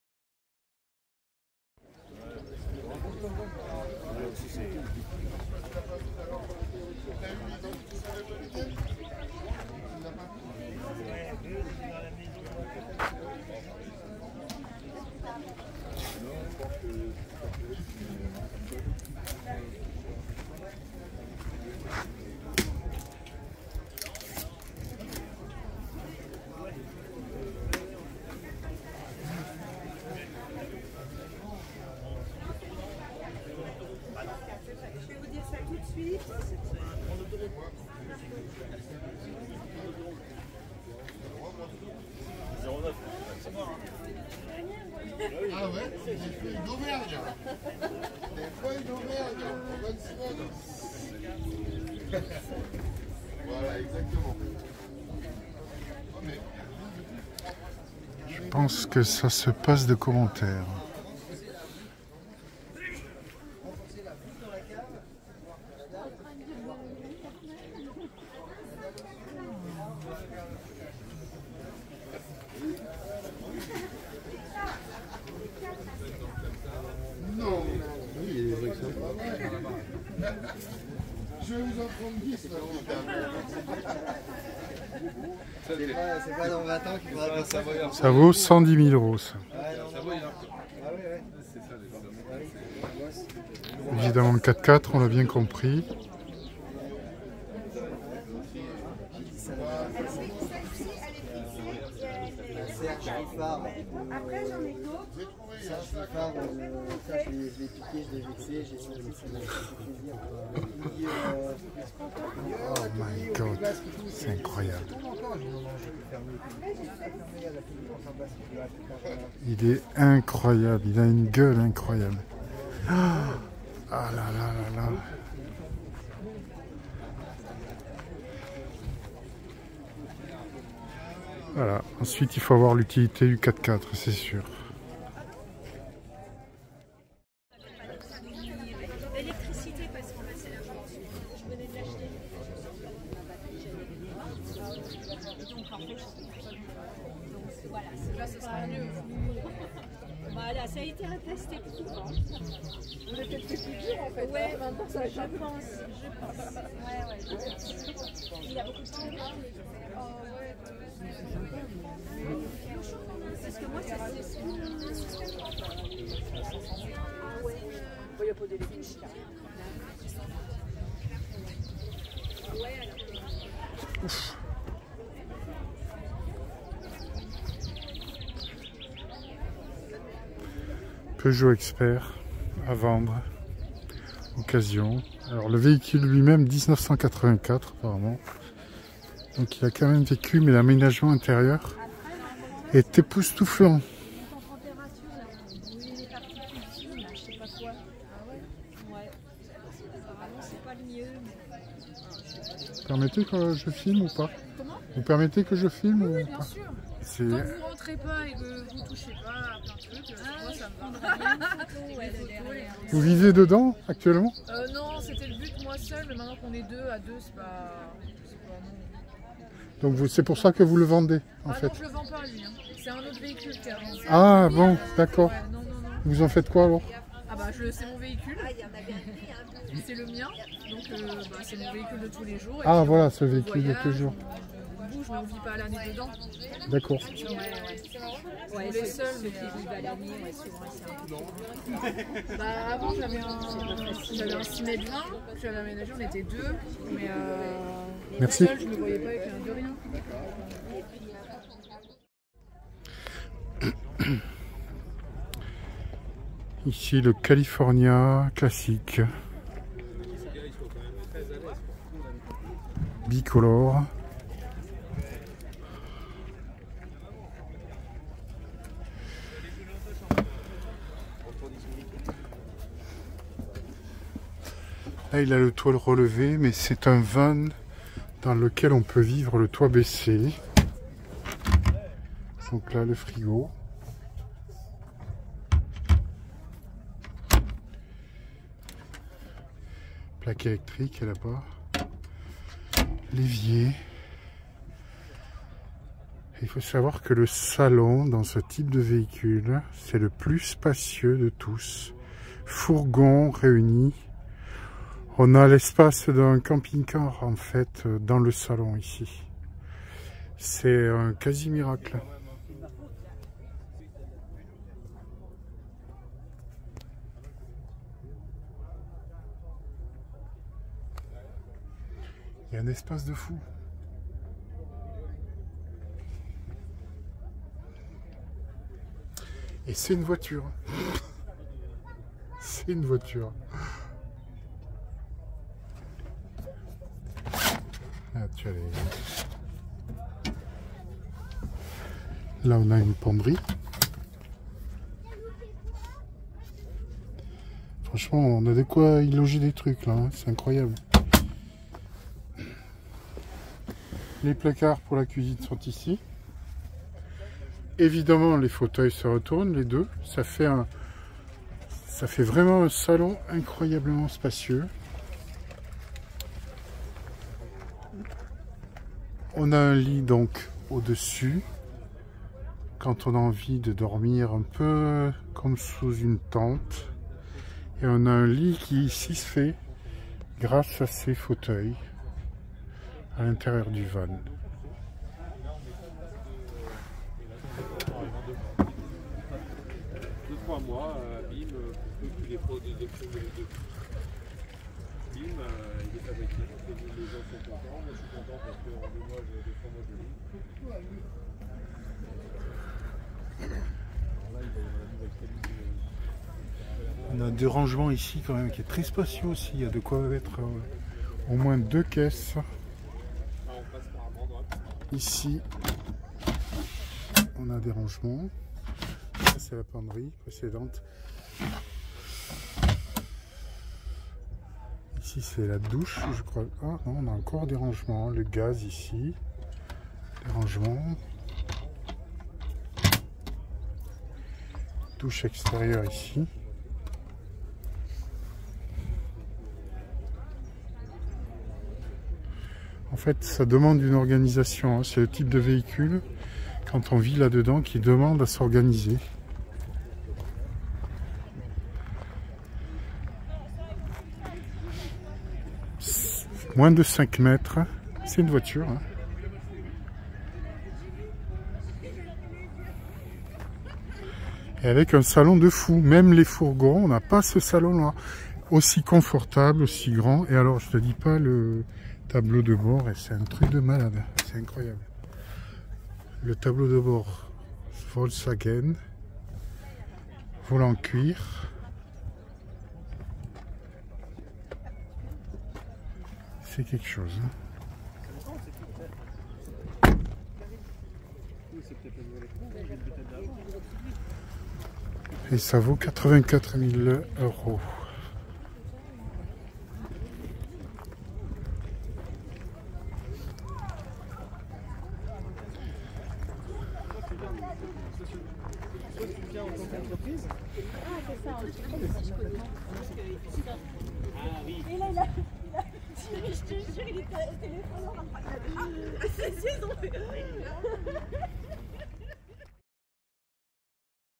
Je me perds dans le choix de mon prochain Van aménagé !
Au moment où je pensais que depuis le we dernier mon choix s'orientait désormais sur un fourgon plutôt qu'un Van, voilà qu'à la suite de l'exposition de ce we-ci le Van California fait dans mon Esprit un retour autant inespéré que spectaculaire !
Je t'explique pourquoi dans cette vidéo.

Chapitrage de cette vidéo :
00:00 4x4 Volkswagen type California
2:18 Aménagement d'un particulier
4:12 Volkswagen California classique